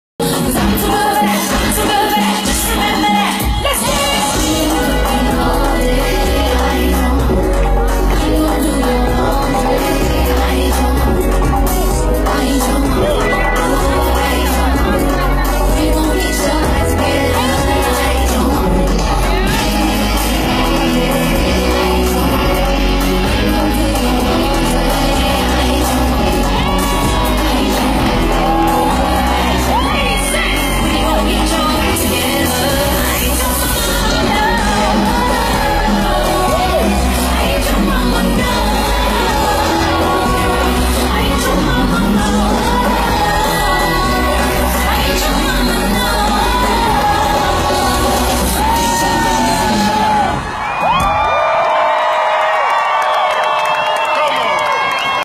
Jennifer lopez dancing for Hillary Clinton
jennifer lopez dancing for Hillary Clinton the next president of the United States